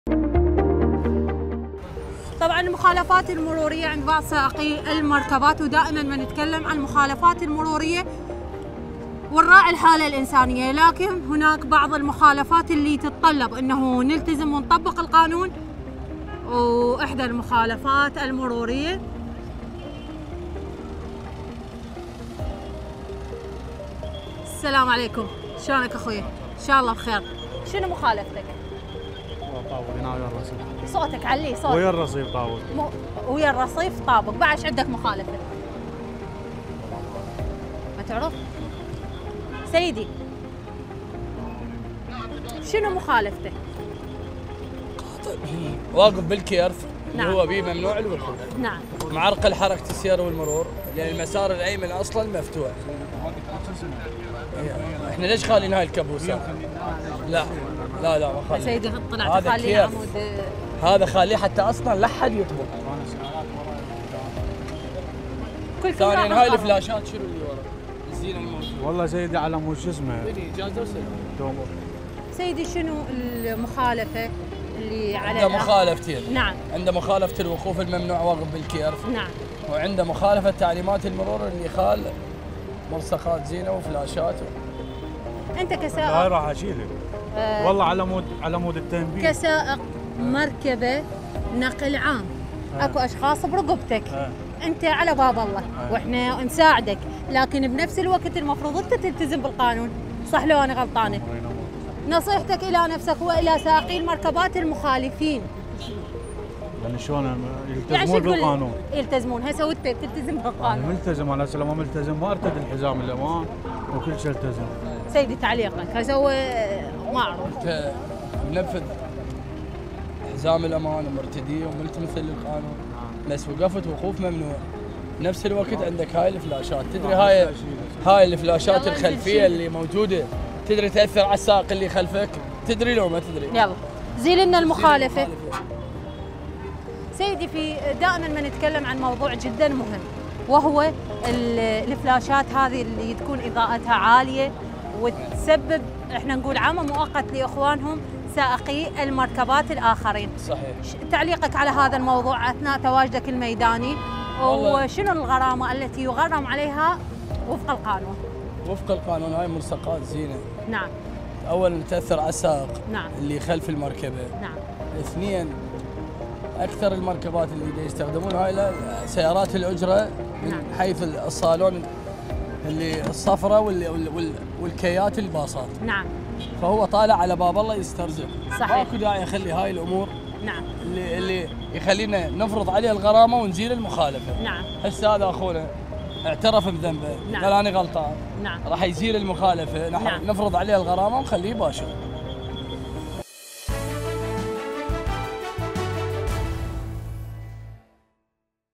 سائق كيا لشرطة المرور : اني "طابق ويا الرصيف" وما عندي مخالفة
موقع قناة التغيير الفضائية  الالكتروني